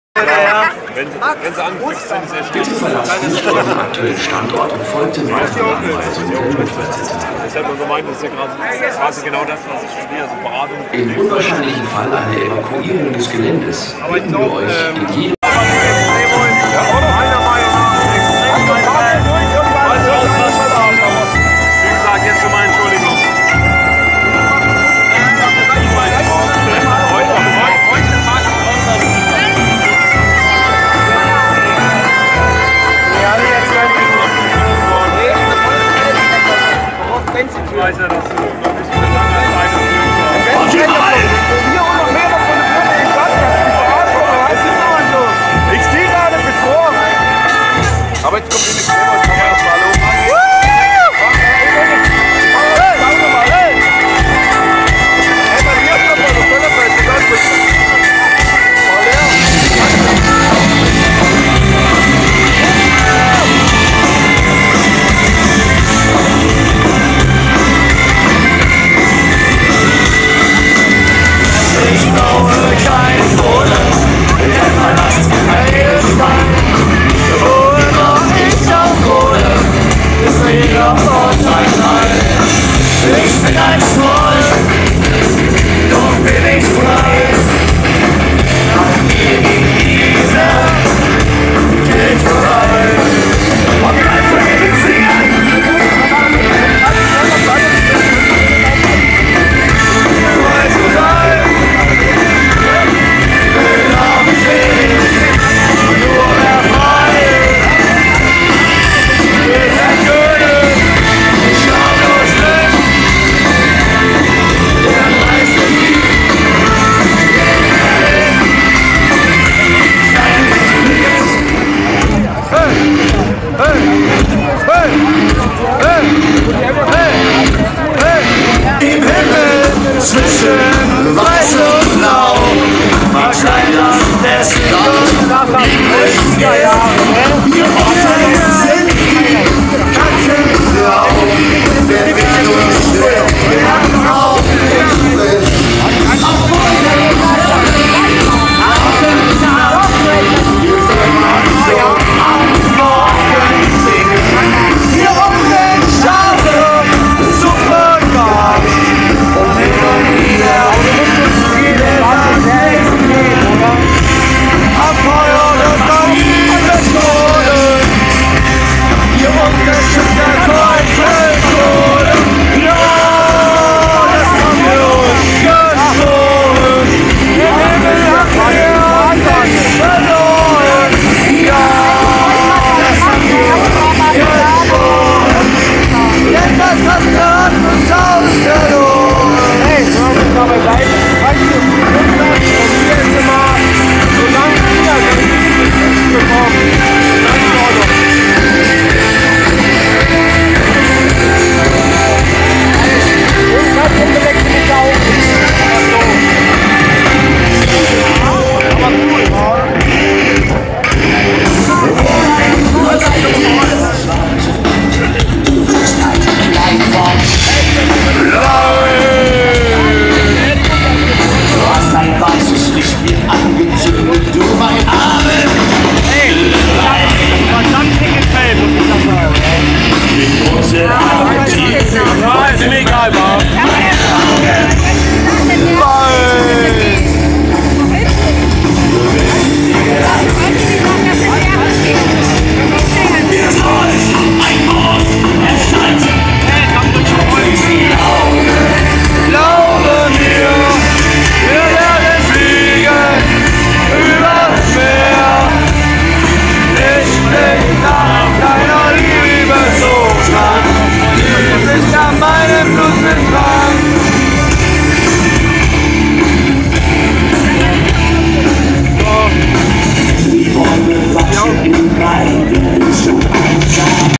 InExtremo live am Hockenheimring 2015 (Best of)
Hir das best of video von inextremo die als vorband von den b�hsen onkelz auf den hhr gespielt haben. Viel Spa�